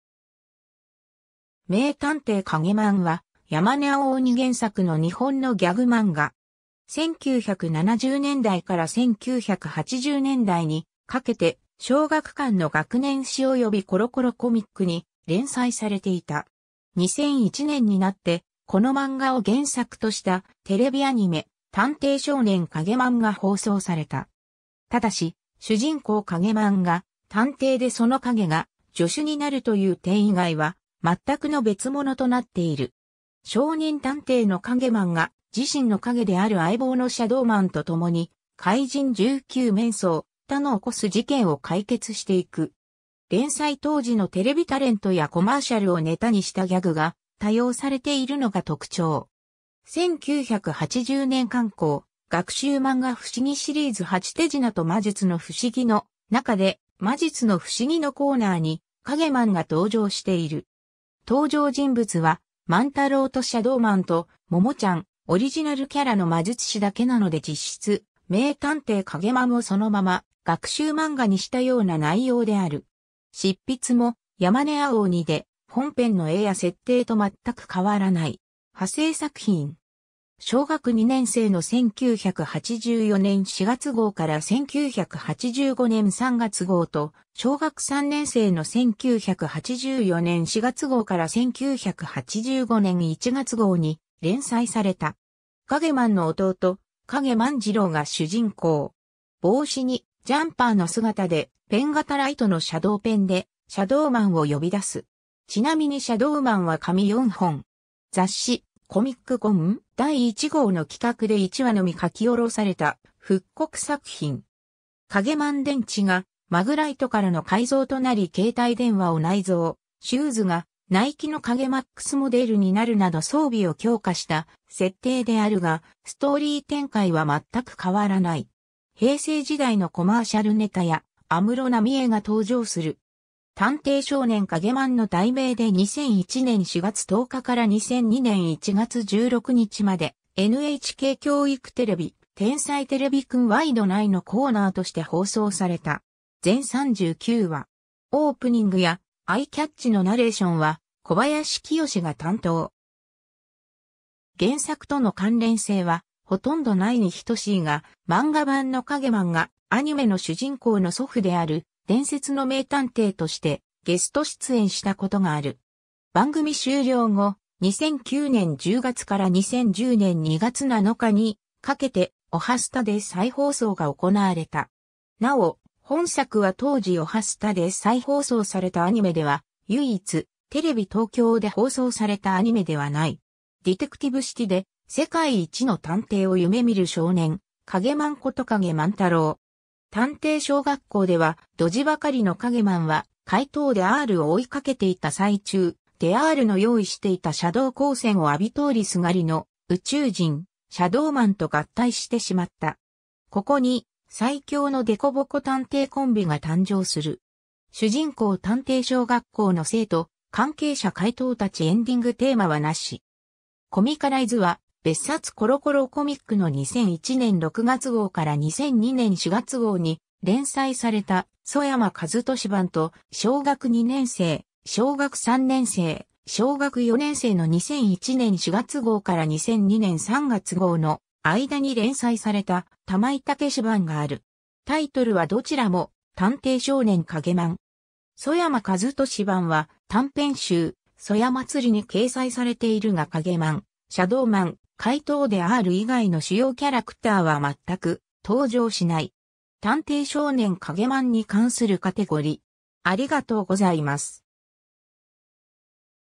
名たんていカゲマン
『名たんていカゲマン』（めいたんていカゲマン）は、山根青鬼（山根あおおに）原作の日本のギャグ漫画。
1970年代から1980年代にかけて小学館の学年誌およびコロコロコミックに連載されていた。
 2001年になって、この漫画を原作としたテレビアニメ『探偵少年カゲマン』が放送された。
ただし、主人公カゲマンが探偵でその影が助手になるという点以外は全くの別物となっている。
 少年探偵のカゲマンが、自身の影である相棒のシャドーマンと共に怪人19面相・他の起こす事件を解決して行く。
連載当時のテレビタレントやコマーシャルをネタにしたギャグが多用されているのが特徴。
 1980年刊行「学習まんが ふしぎシリーズ8 手品と魔術のふしぎ」の中で、「魔術のふしぎ」のコーナーにカゲマンが登場している。
登場人物は万太郎とシャドーマンとモモちゃん、オリジナルキャラの魔術師だけなので実質「名たんていカゲマン」をそのまま学習まんがにしたような内容である。
執筆も山根あおおにで、本編の絵や設定とまったく変わらない。
 派生作品。
小学二年生の1984年4月号から1985年3月号と、小学三年生の1984年4月号から1985年1月号に連載された。
カゲマンの弟、影万次郎（通称は兄と同じくカゲマン）が主人公。
帽子にジャンパーの姿で、ペン型ライトのシャドーペンでシャドーマンを呼び出す。
ちなみにシャドーマンは髪4本。
 雑誌「COMIC GON! 第1号（1997年11月、ミリオン出版）」の企画で1話のみ書き下ろされた復刻作品。
カゲマン電池がマグライトからの改造となり携帯電話を内蔵、シューズがナイキのカゲMAXモデルになるなど装備を強化した設定であるが、ストーリー展開はまったく変わらない。
平成時代のコマーシャルネタや『アムロなみえ』が登場する。
 『探偵少年カゲマン』の題名で2001年4月10日から2002年1月16日までNHK教育テレビ『天才てれびくんワイド』内のコーナーとして放送された。
全39話。
オープニングやアイキャッチのナレーションは小林清志が担当。
 原作との関連性はほとんどないに等しいが、漫画版のカゲマンがアニメの主人公の祖父である「伝説の名探偵」としてゲスト出演したことがある。
 番組終了後、2009年10月から2010年2月7日にかけて『おはスタ』で再放送が行われた。
なお、本作は当時おはスタで再放送されたアニメでは唯一、テレビ東京で放送されたアニメではない。
 ディテクティブ・シティで世界一の探偵を夢見る少年、カゲマンこと影万太郎。
探偵小学校ではドジばかりのカゲマンは、怪盗デ・アールを追いかけていた最中、デ・アールの用意していたシャドー光線を浴び通りすがりの宇宙人シャドーマンと合体してしまった。
ここに最強の凸凹探偵コンビが誕生する。
 主人公 探偵小学校の生徒・関係者 怪盗たち エンディングテーマはなし。
 コミカライズは、別冊コロコロコミックの2001年6月号から2002年4月号に連載された曽山一寿版と、小学二年生・小学三年生・小学四年生の2001年4月号から2002年3月号の間に連載された玉井たけし版がある。
タイトルはどちらも『探偵少年カゲマン』。
 曽山一寿版は短編集「そやまつり」に掲載されているがカゲマン、シャドーマン、怪盗デ・アール以外の主要キャラクターはまったく登場しない。
 探偵少年カゲマンに関するカテゴリ: